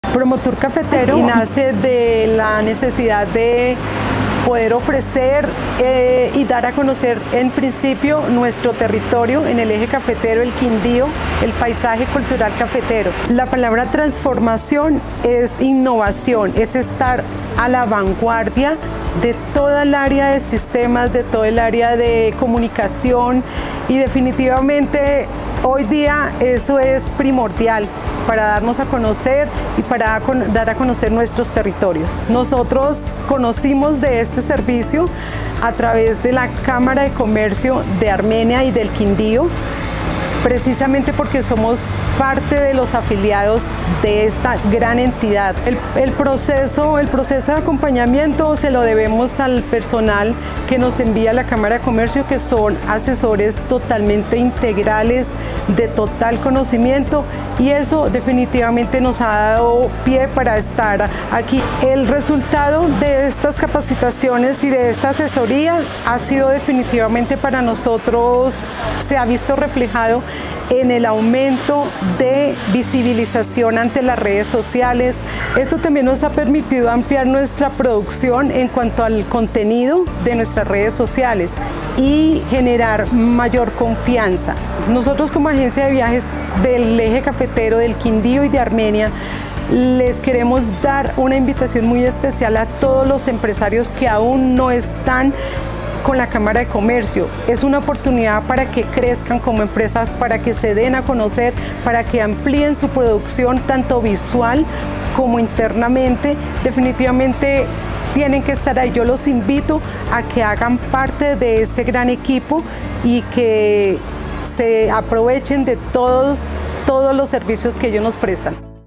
Promotour Cafetero: Cómo la transformación digital impulsó su crecimiento
¡No te pierdas este caso de éxito empresarial! 💪🏻🚀 Promotour Cafetero se transformó digitalmente gracias al acompañamiento de la Cámara de Comercio de Armenia y del Quindío mejorando su presencia en línea y llegando a nuevos clientes.

¿Quieres saber cómo lo lograron? ¡Dale play al video! ▶️

Tu también puedes acceder sin costo a este servicio 👉🏻 Escríbenos por Whastapp para darte más información 📲➡️